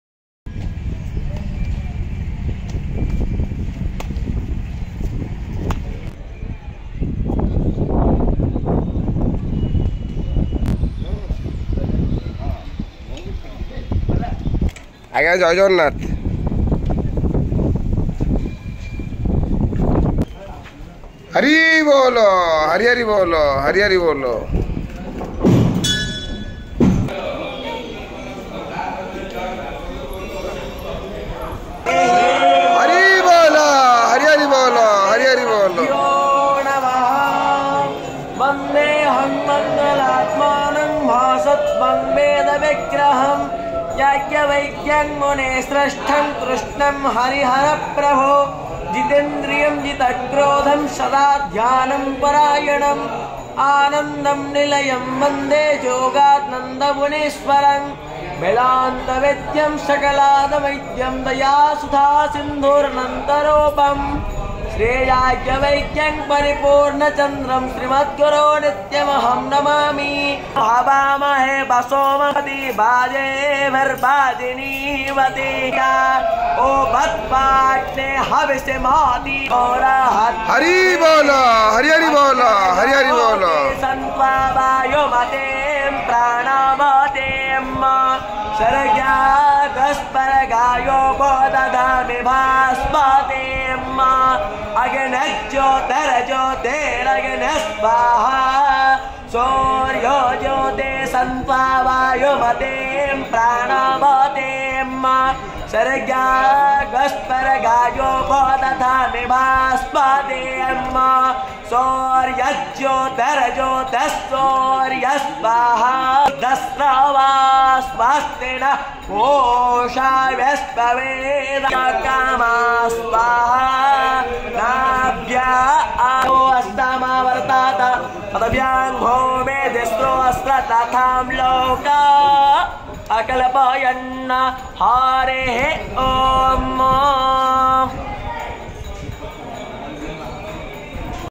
Sinha Dwar opening rituals Patitapaban Mangal Arati Darshan Sri Jagannath Temple, Puri Dt.02/04/2022
🙏ଜୟ ଜଗନ୍ନାଥ

ସିଂହଦ୍ୱାର ଫିଟା ନିତୀ ଭିତରଛ ମହାପାତ୍ର ଙ୍କ ଦ୍ୱାରା 04.50AM ରେ ସମାପ୍ତ ହେଲା

🙏Jay Jagannath 🙏

|| Jay Jagannath ||